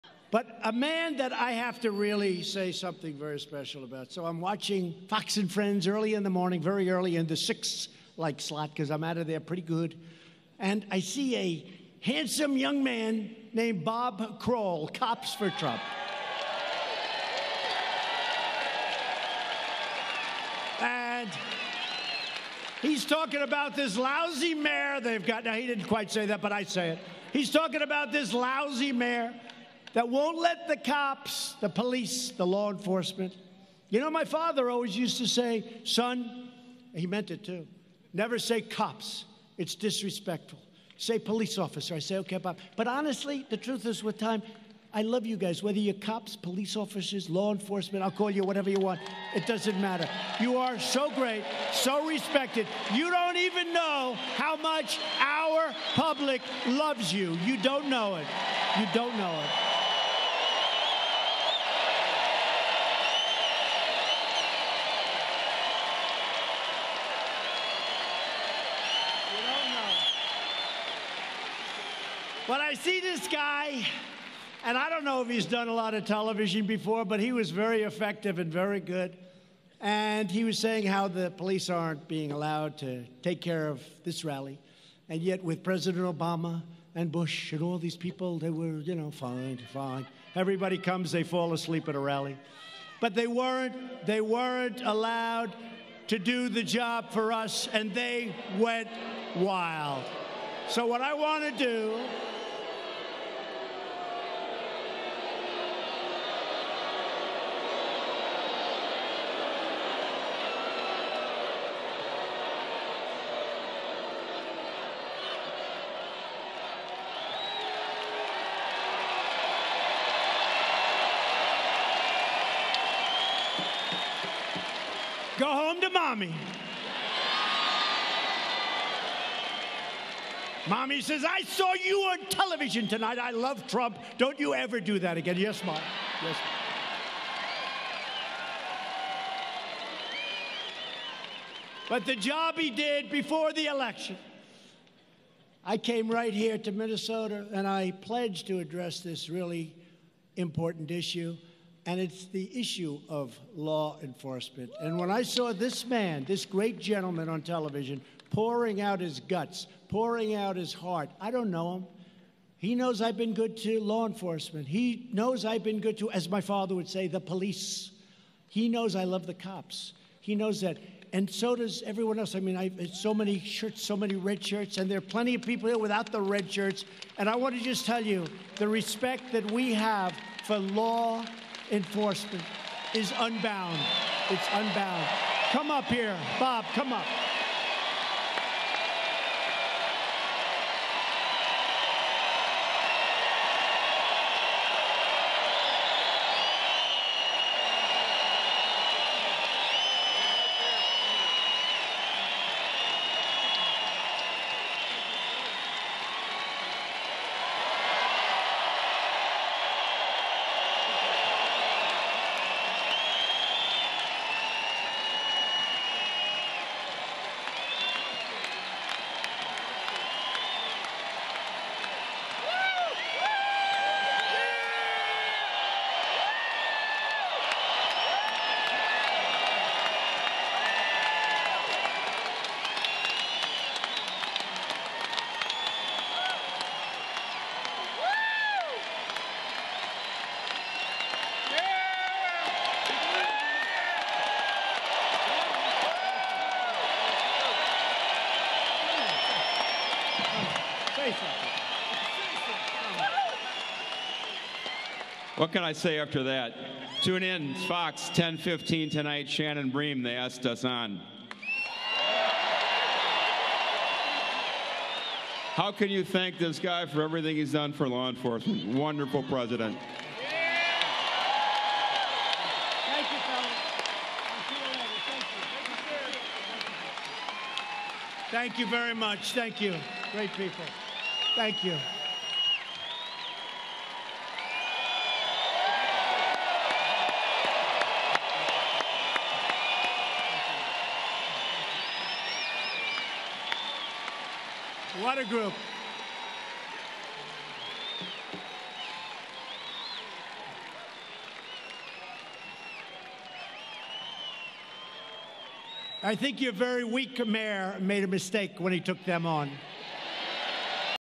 President Donald Trump Invites Minneapolis PD Onstage at Rally
President Donald Trump introduces Minneapolis Police Union President Lt. Bob Kroll and several police officers of the Minneapolis Police Department onstage at this October 10, 2019, rally in Minneapolis. The president of the union has asserted that officer Derek Chauvin, George Floyd’s killer, was not onstage at the rally.